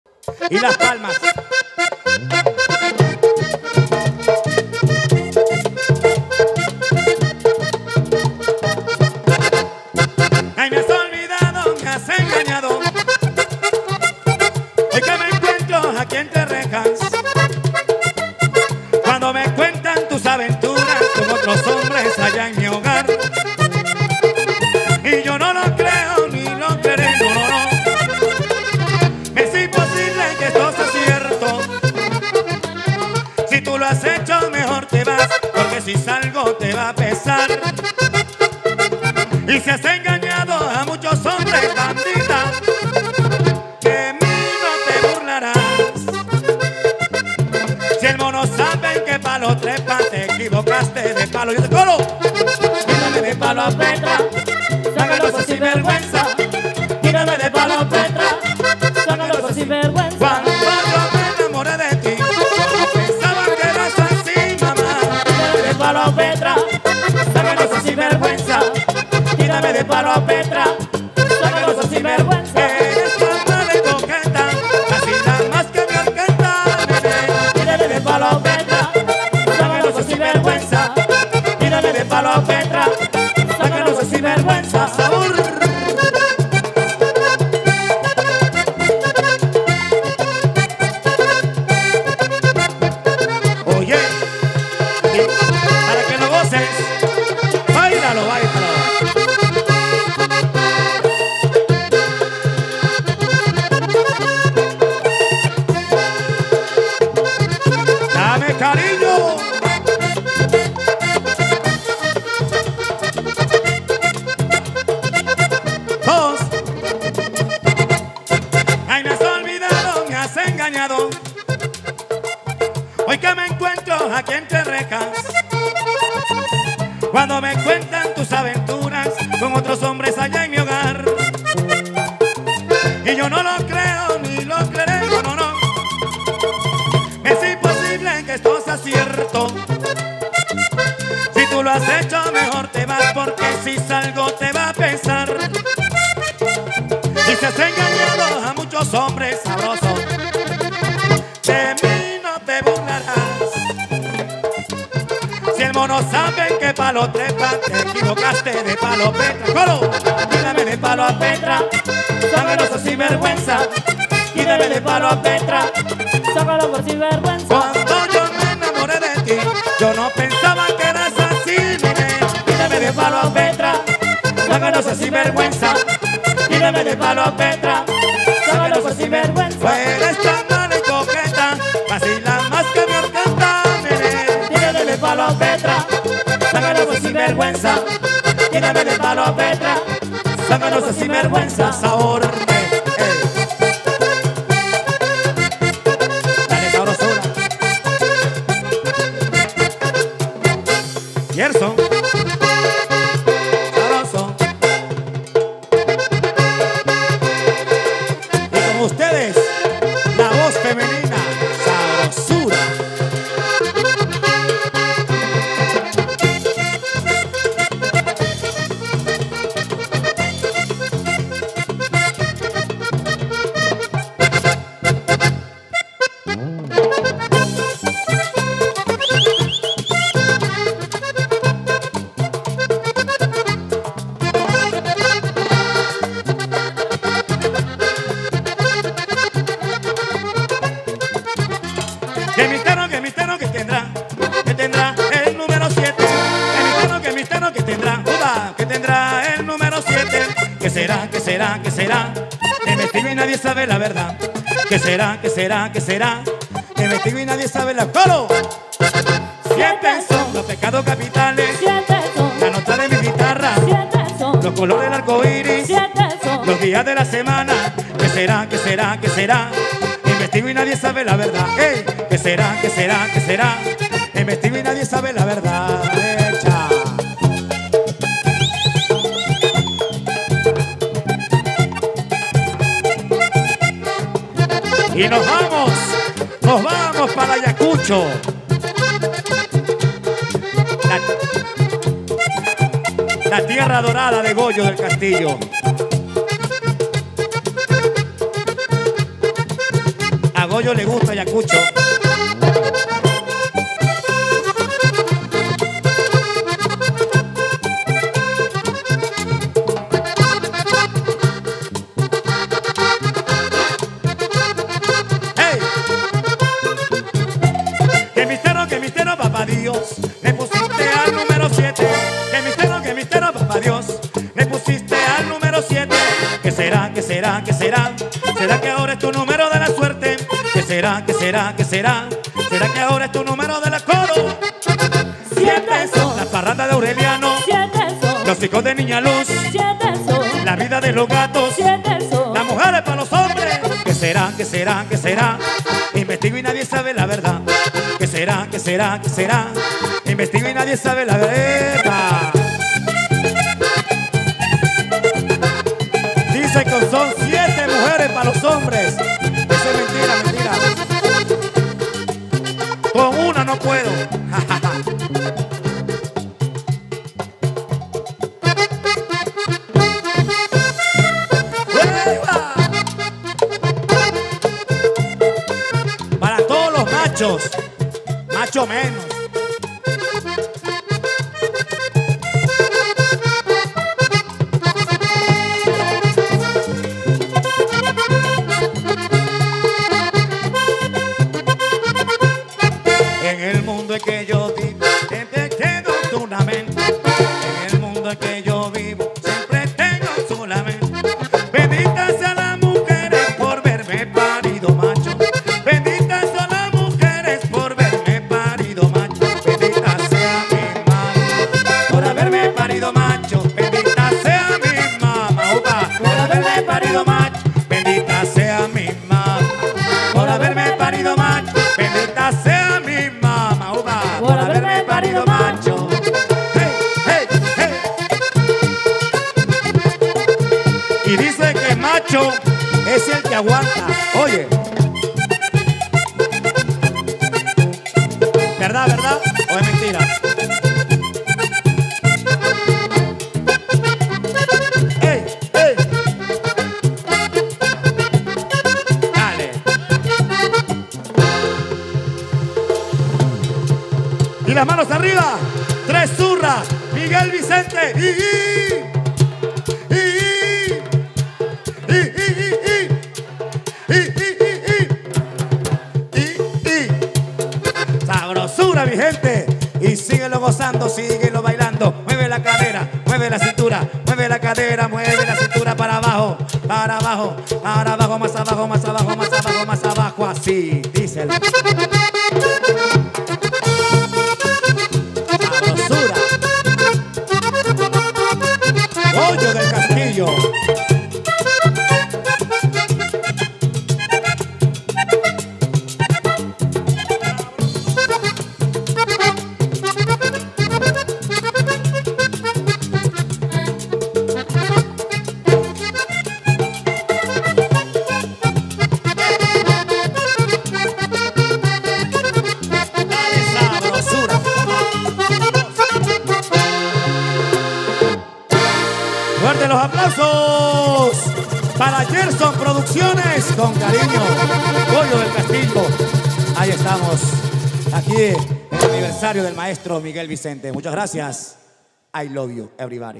MIX LISANDRO MEZA - LOS CONSENTIDOS DEL VALLENATO / HOMENAJE A MIGUEL VICENTE - VILLA SALVADOR 2023
CONTRATOS ARTISTAS Ó ORQUESTAS  Y VIDEOS EN VIVO /AUDIOVISUALES /FILMACIONES Y VIDEOCLIP: 100% GARANTIZADO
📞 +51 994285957
PRODUCCION AUDIOVISUAL GERSON SALSA